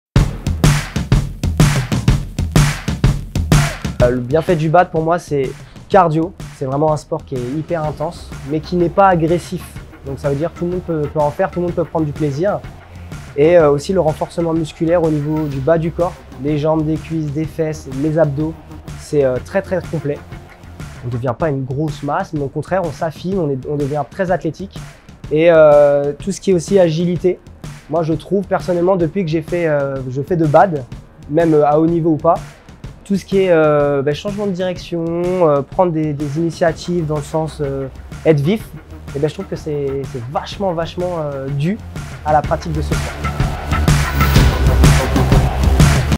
TEAM MMSPORT 2024 : « Les bienfaits de ton sport ? » Méril Loquette - Episode 4
Nouvel épisode ! Découvrez la discipline de Méril Loquette !

Dans ce quatrième épisode, découvrez les bienfaits du badminton avec Méril Loquette !

Plongez dans l'univers captivant du badminton aux côtés de Méril Loquette dans cette vidéo exclusive. Découvrez les différents bienfaits du badminton, l'agilité, le renforcement musculaire ou encore la coordination et la réactivité nécessaire pour ce sport.
Rejoignez-nous dans cette aventure sportive et motivante avec Méril Loquette.